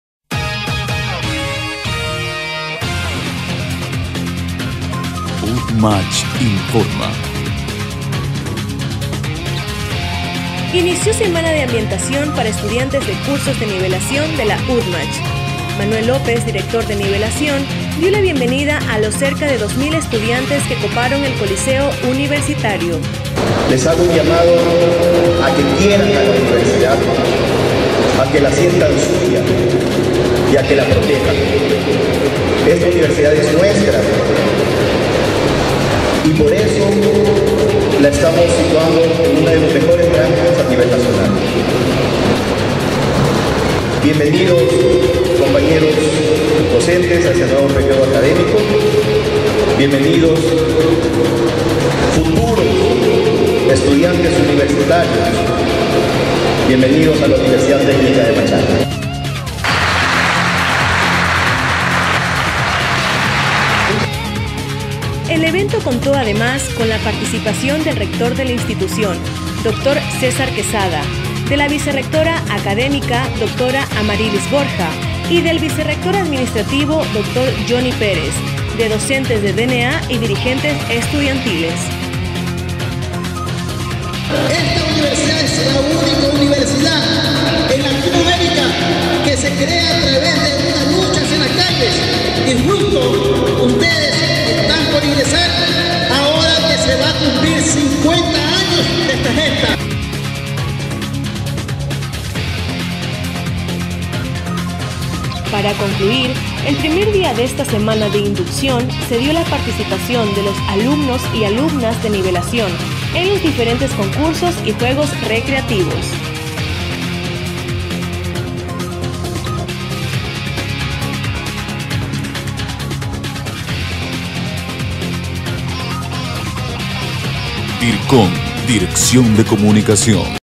SEMANA DE INDUCCIÓN A ESTUDIANTES DE NIVELACIÓN
Este martes 24 de abril, en el coliseo universitario, inició la Semana de Inducción a los estudiantes de los cursos de nivelación, con un acto de bienvenida en el que participaron las principales autoridades de nuestra universidad.